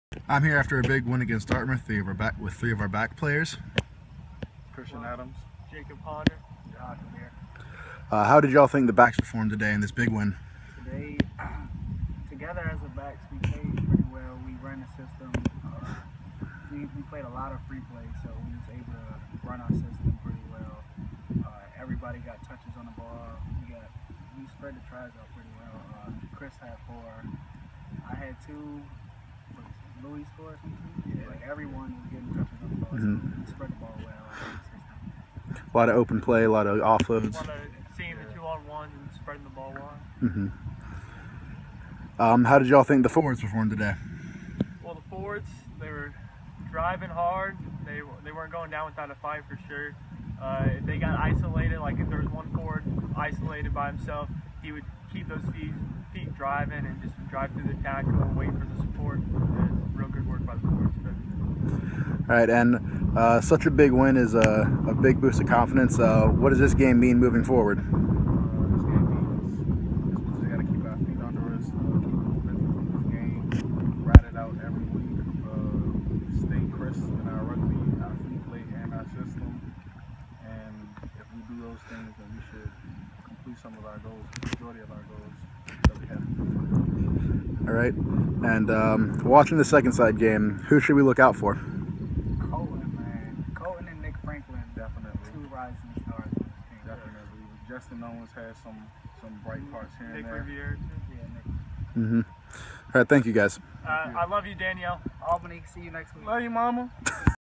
Post Game Interview Dartmouth
Checking in with a few of the backs (Christian Adams, Jacob Ponder, Jihad Khabir) after a 79-14 win over Dartmouth.